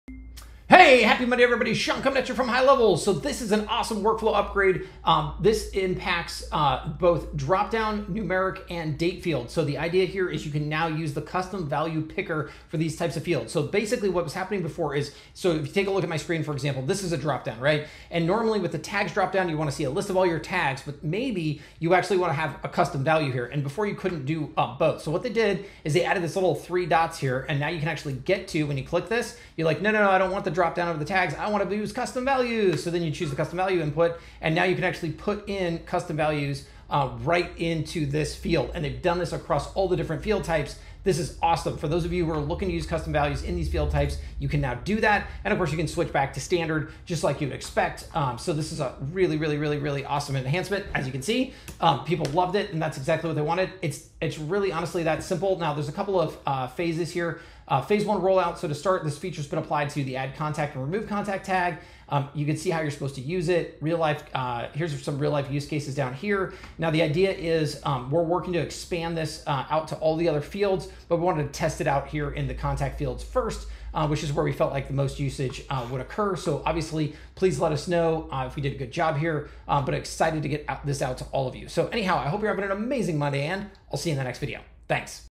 Custom Value Picker for Dropdown, Numeric and Date Fields!
Custom Value Picker for Dropdown, Numeric and Date fields :fire::fire:
Phase 2
For the second phase of the release we have added the same functionality to 2 more actions:
1.- Assign User - This will support User ID. The User IDs that are added can be string ‘user-1’ or [‘user-1’, ‘user-2’], or ‘user-1, user-2’
2.- Copy Contact